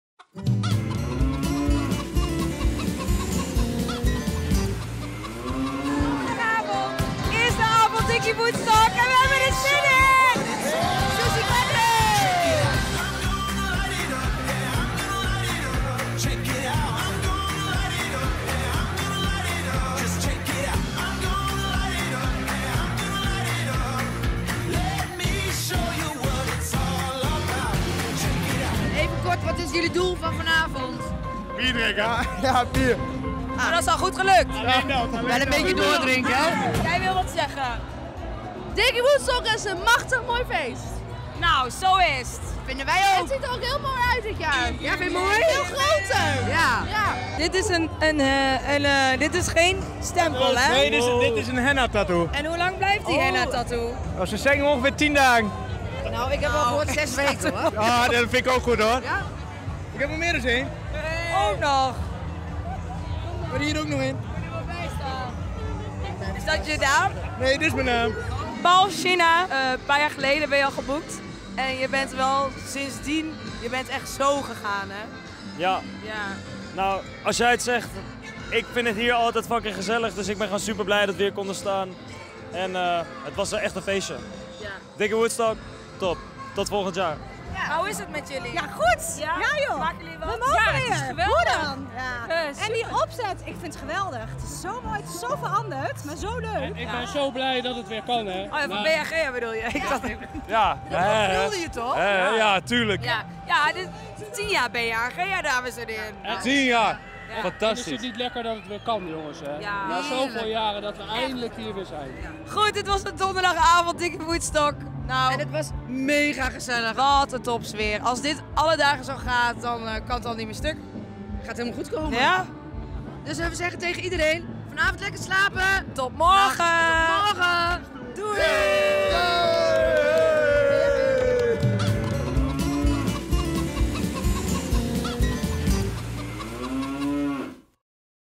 Dicky Woodstock 2022 - Donderdagavond
Het feest barst los, Bea & Gea zoeken de woodstockgangers op om mee te feesten, te kletsen en te horen hoe het is om EINDELIJK weer naar het Dicky Woodstock popfestival te mogen gaan.